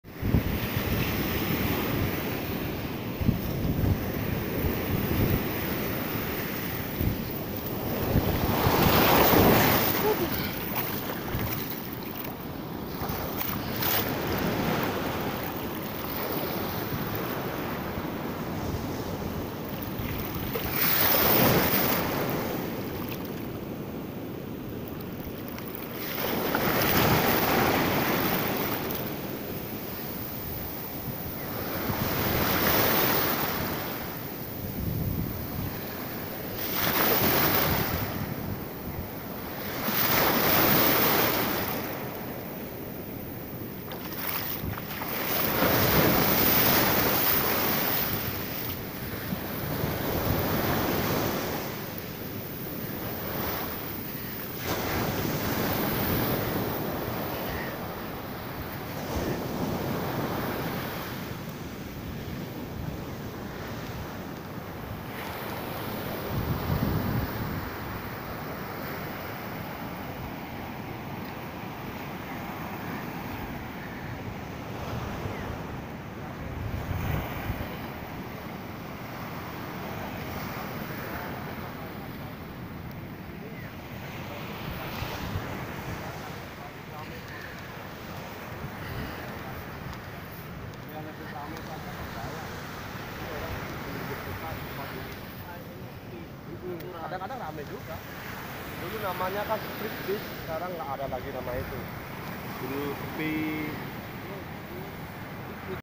Bali Pantai dsini tdk tau nam pantai kota nah
Salam 🙏 tolong aku, Help me,
saya lupa pantai Bali disini, bisa saya minta nama pantainya, itu adalah pantai yg dihapus dari kota. makasih  :)